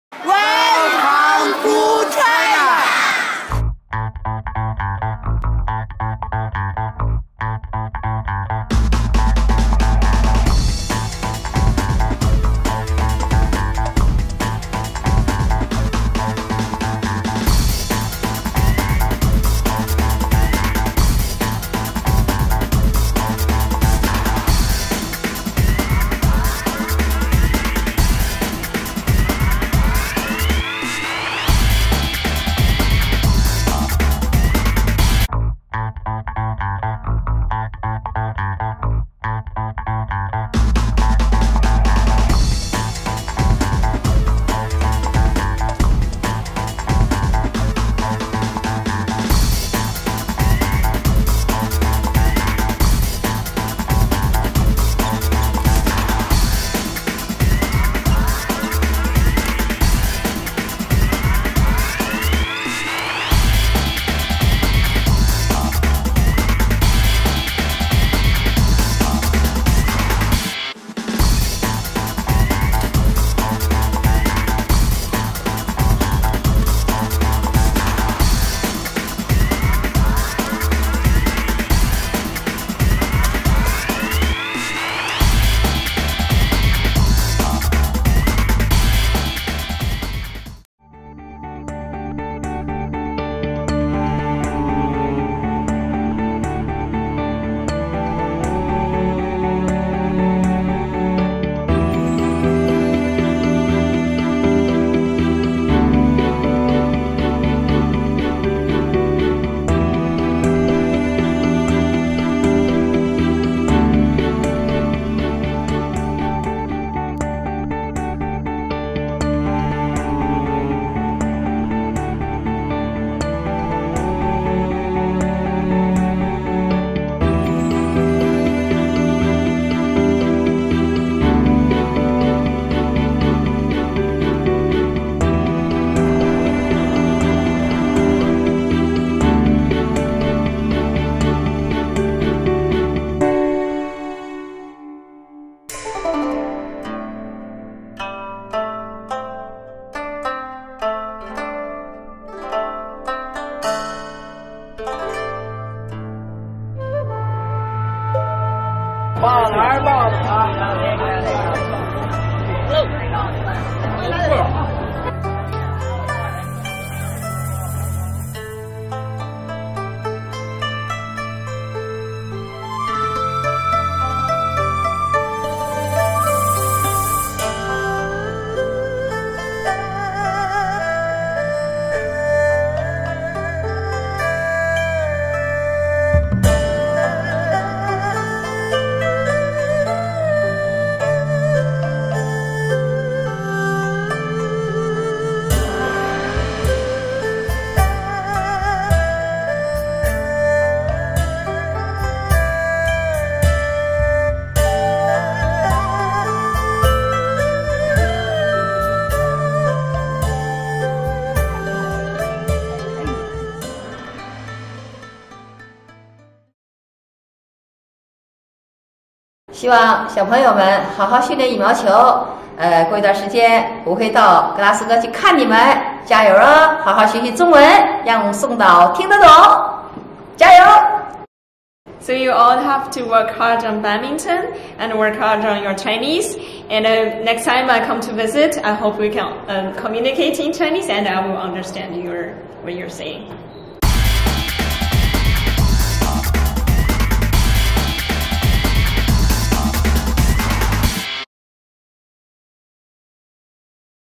Glasgow School of Sport Badminton Exchange to Dalian, China
Glasgow School of Sport has just set up a badminton exchange with Dalian in China. In October ten players and two coaches will travel there to train alongside some top junior players from Liaoning province and experience the food, culture and traditions of such an amazing place.  This video captures some of what they can expect.